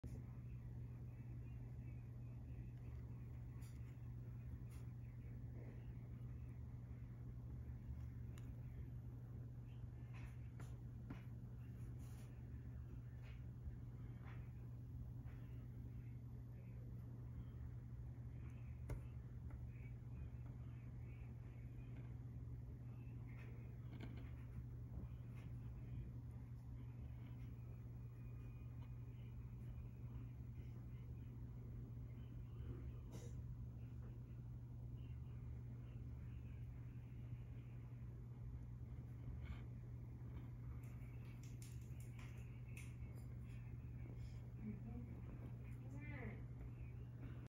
Tetramorium immigrans Colony A, ‘STORM’ 10/10/‘24
My main Tetramorium immigrans colonies. Has around 200 workers.